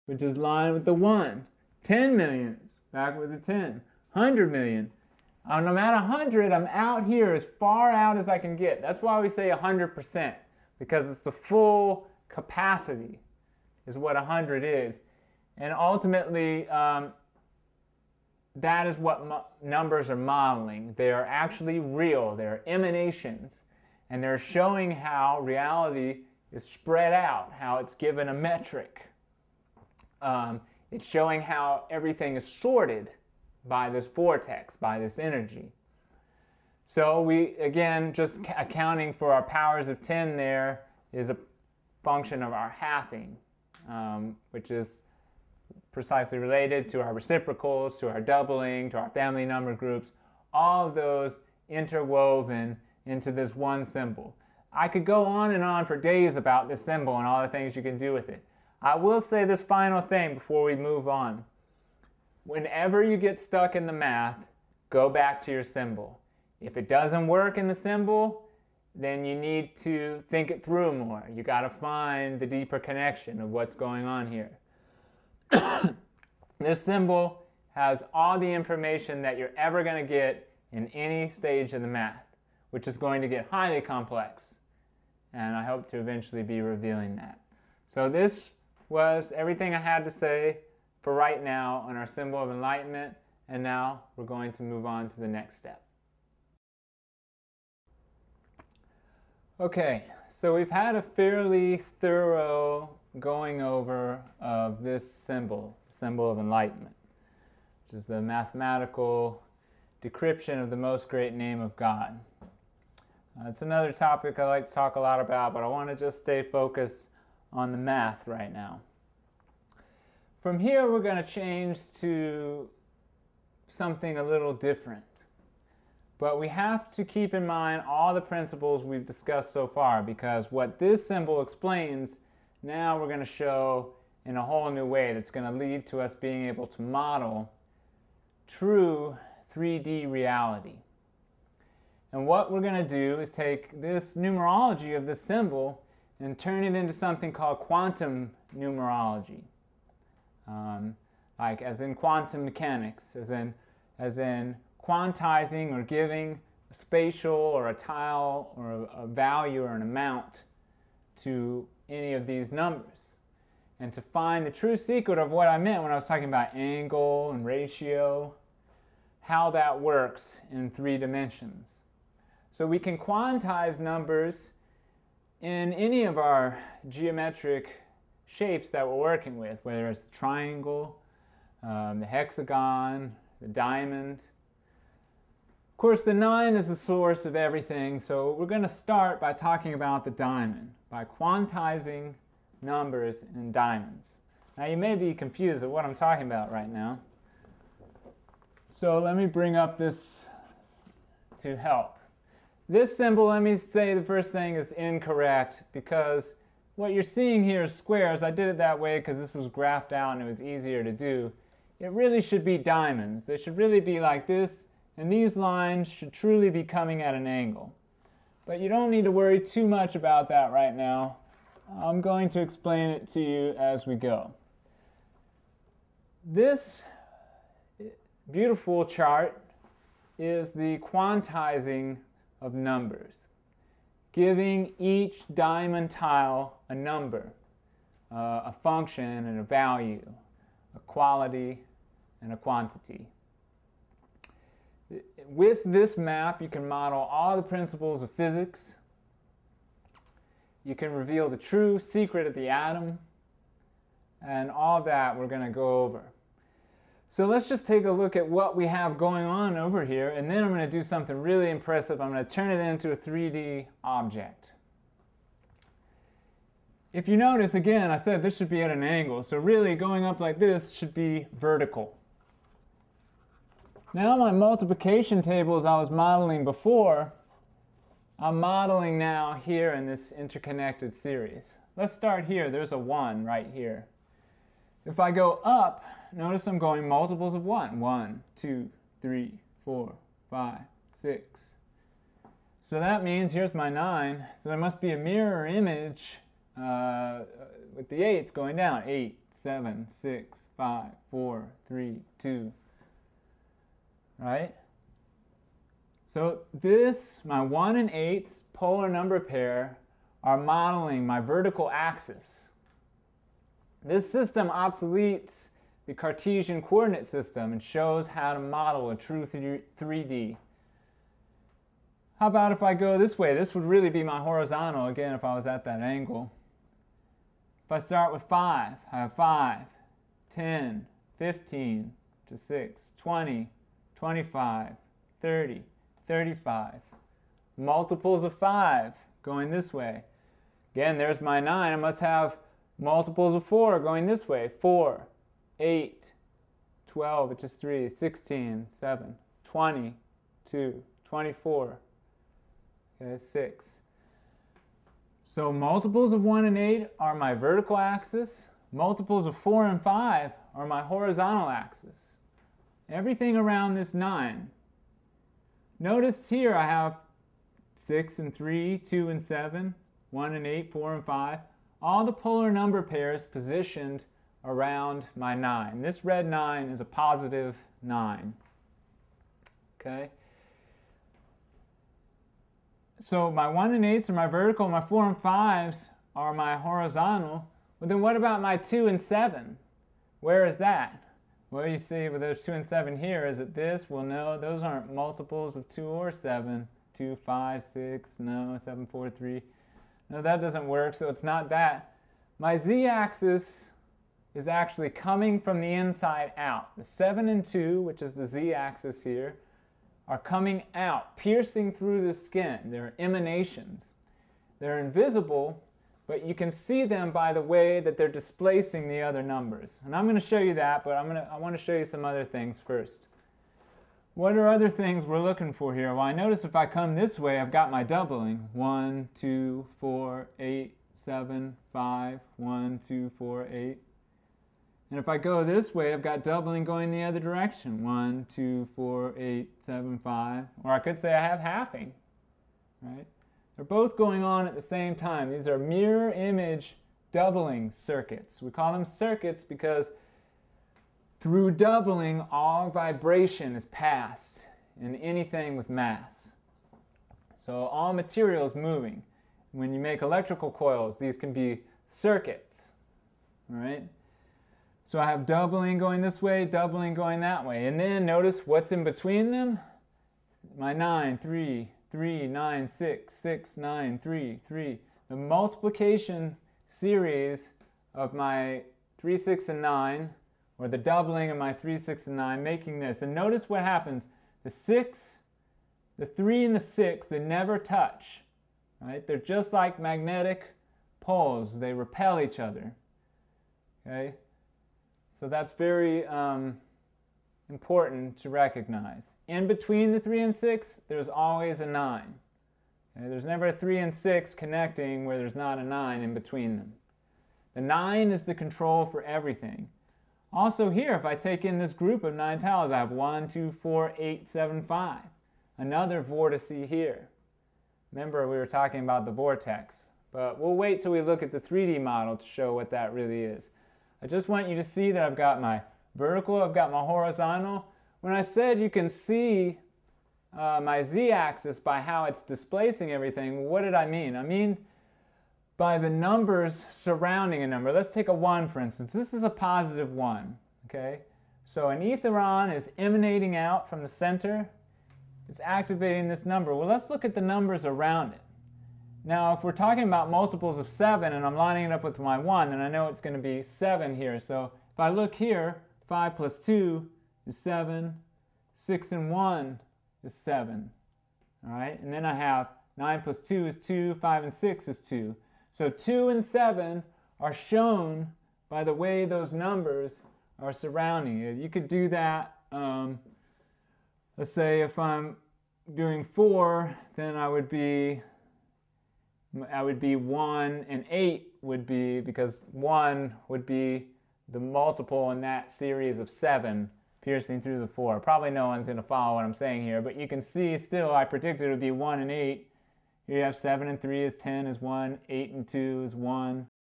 Randy Powell - Intro to Vortex Math - Part 9
Randy Powell explains vortex math and the ABHA Torus from the beginning.